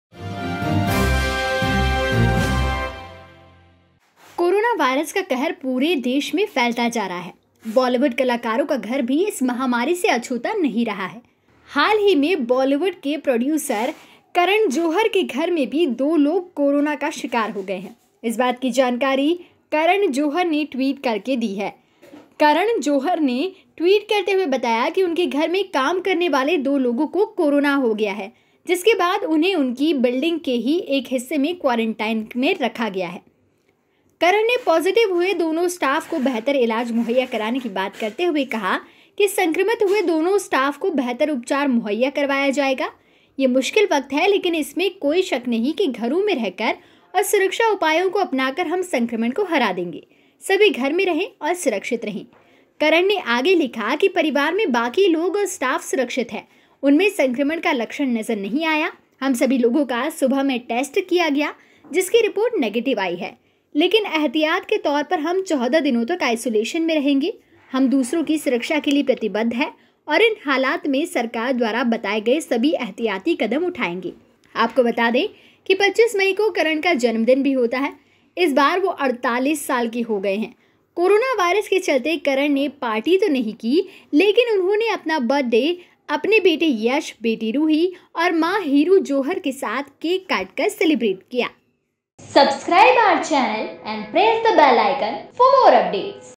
Corona की चपेट में आया Bollywood Director Karan Johar का घर, खुद को किया Isolate
करण जोहर के स्टाफ के 2 लोग कोरोना संक्रमित
करण जौहर ने ट्वीट कर  दी जानकारी

कोरोना वायरस  का कहर पूरे देश में फैलता ही जा रहा है बॉलीवुड कलाकारों का घर भी इस महामारी से अछूता नहीं रहा है अब हाल ही में बॉलीवुड के प्रोड्यूसर करण जौहर के घर में भी दो लोग कोरोना का शिकार हो गए हैं इस बात की जानकारी करण जौहर ने ट्वीट करके दी है करण जौहर  ने ट्वीट करते हुए बताया कि उनके घर में काम करने वाले दो लोगों को कोरोना हो गया है जिसके बाद उन्हें उनकी बिल्डिंग के ही एक हिस्से में क्वारंटीन में रखा गया है

करण ने पॉजिटिव हुए दोनों स्टाफ को बेहतर इलाज मुहैया कराने की बात करते हुए कहा ki ‘संक्रमित हुए दोनों स्टाफ को बेहतर उपचार मुहैया करवाया जाएगा। यह मुश्किल वक्त है, लेकिन इसमें कोई शक नहीं है कि घरों में रहकर और सुरक्षा उपायों को अपनाकर हम संक्रमण को हरा देंगे। सभी घर में रहें और सुरक्षित रहें।